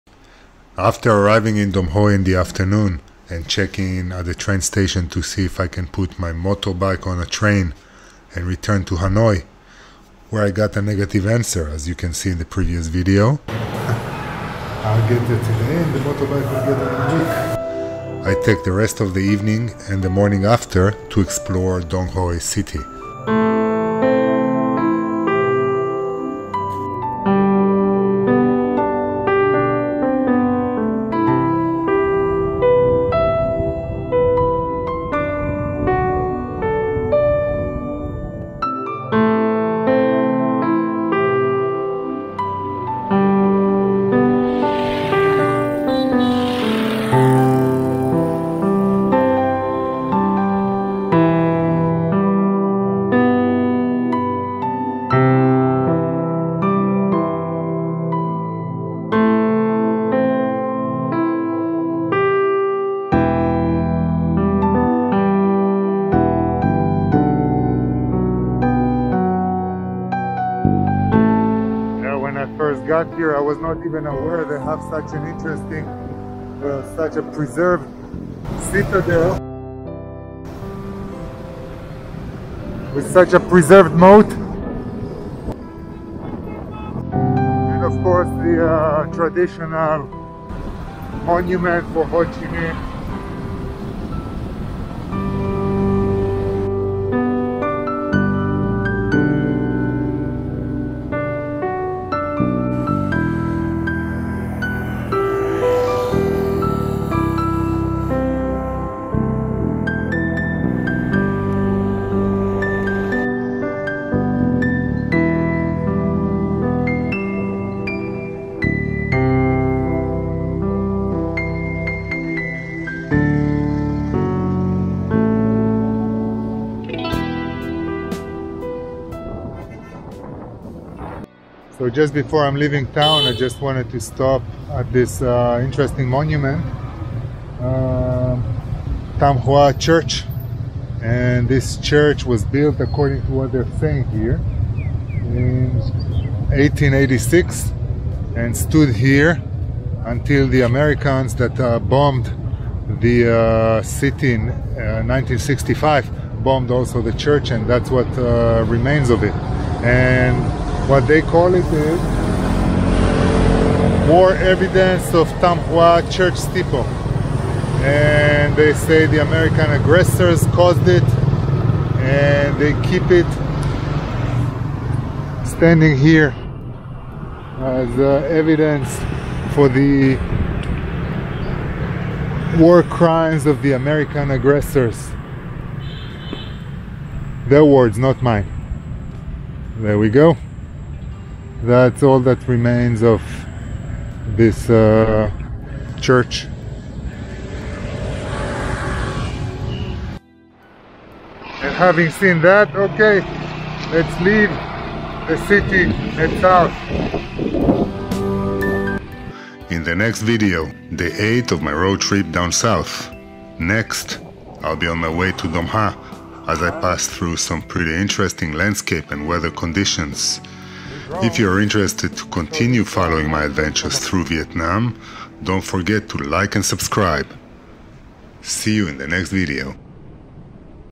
A visit to Dong Hoi
During my motorbike road trip south from Hanoi to Hoi-An, I arrived at the evening of the 7th day of the trip to Dong Hoi  and checked in the local train station if I could put my motorbike on the train and head back to Hanoi together with it. (as can be seen in the previous video I posted) It wasn't possible for the motorbike to arrive in the same day with me, or even in the same week, so I gave up the idea and decided to continue to Dong Ha, more to the south, and try again. In the meantime, I took the evening and the morning after to explore the small and beautiful city of Dong Hoi for a while, before I kept heading south.